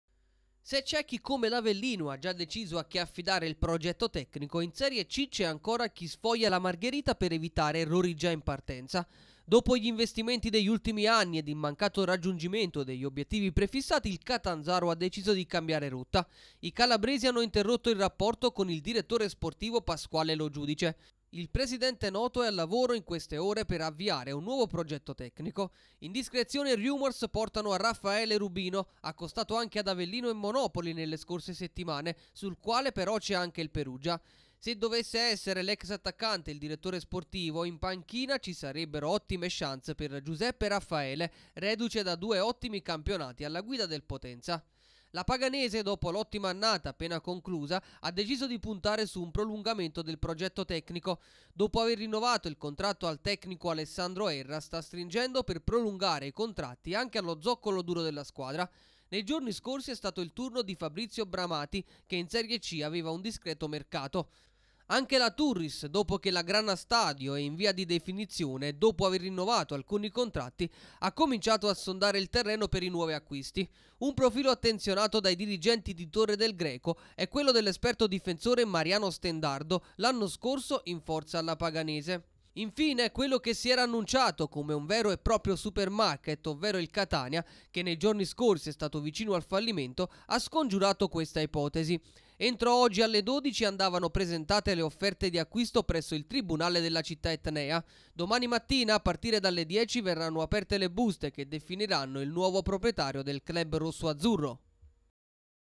Mercato di C, per il Catanzaro pronta la coppia Rubino - Raffaele
Il club calabrese è al lavoro per avviare il nuovo progetto tecnico. Attive anche Paganese e Turris. Il Catania pronto a scoprire il proprio futuro.